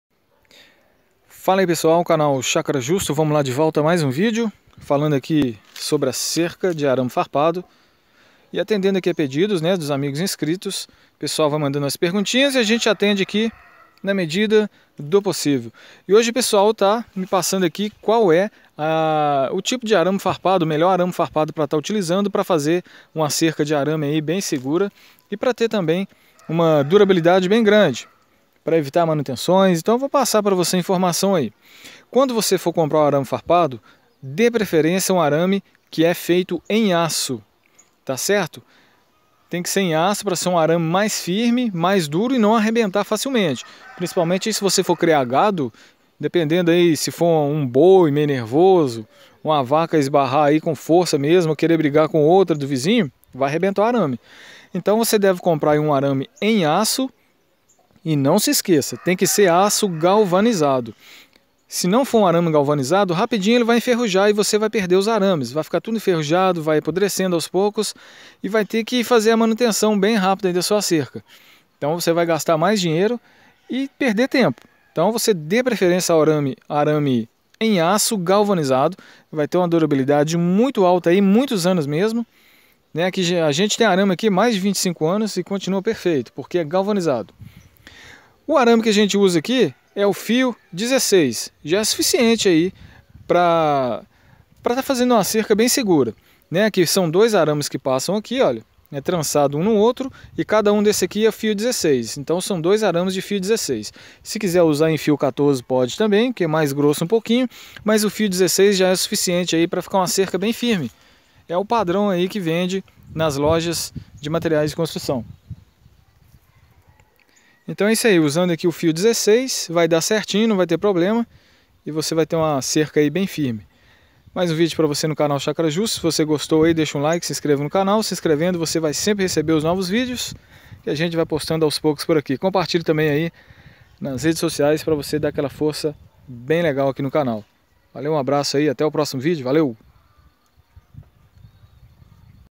Qual é o melhor tipo de arame farpado usar para construir fazer cercas e tapumes? Dica como escolher
. CONFIRA O NOSSO TIKTOK